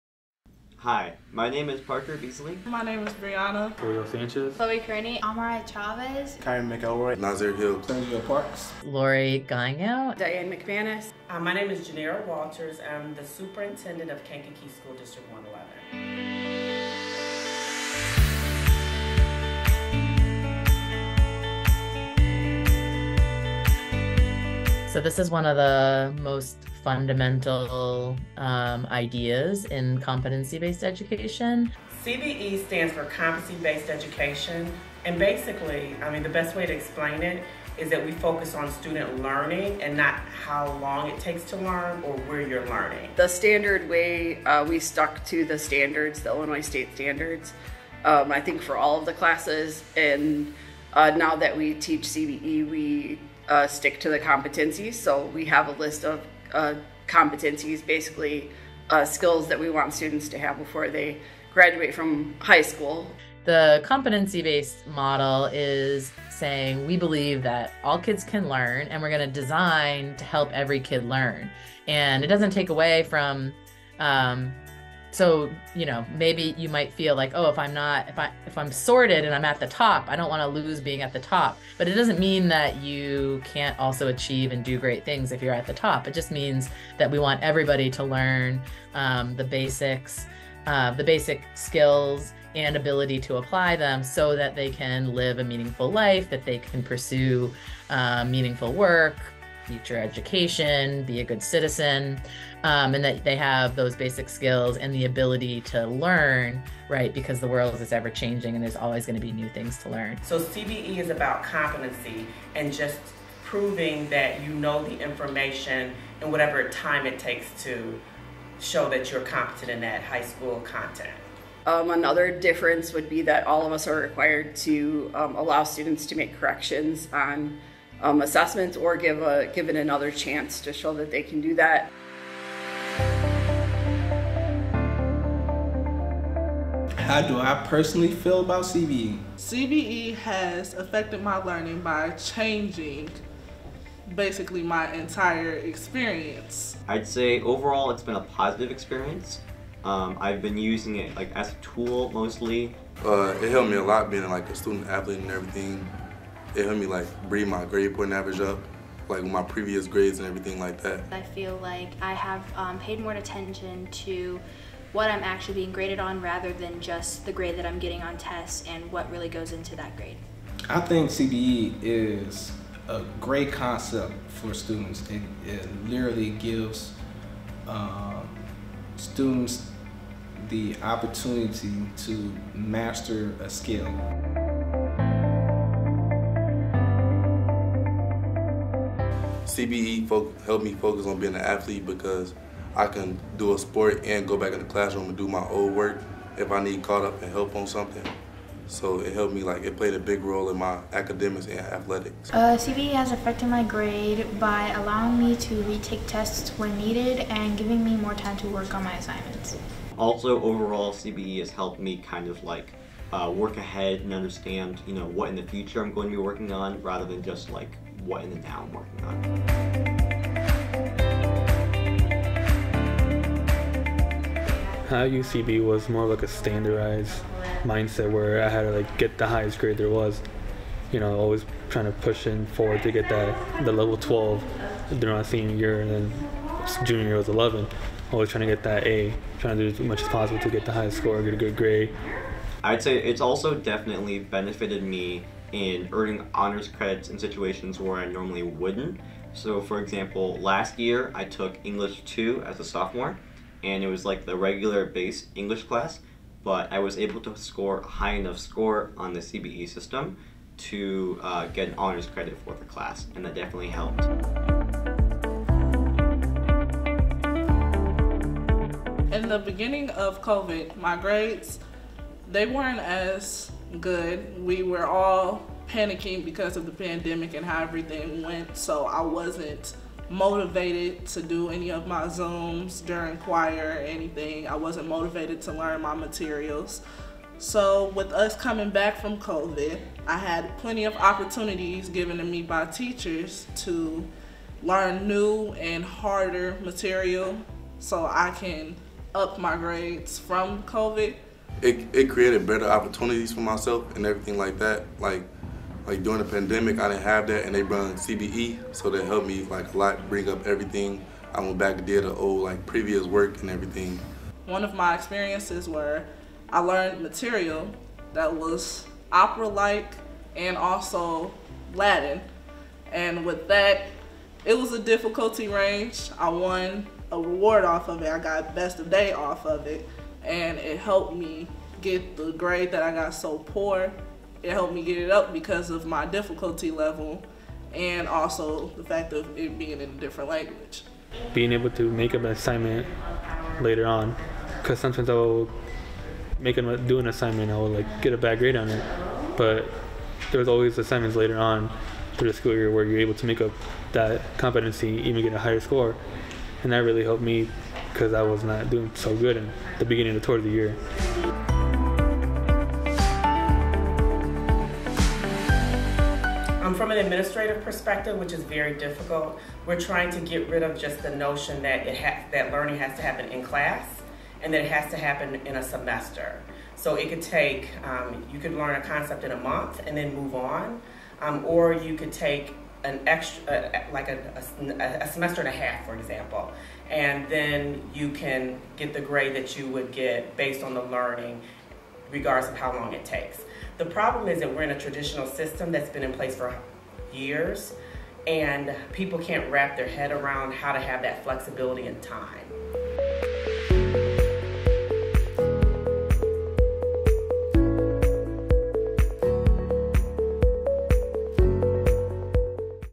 Kankakee High School Docuseries: CBE
A docuseries filmed by Kays Media talking about what CBE is and how the users think of it.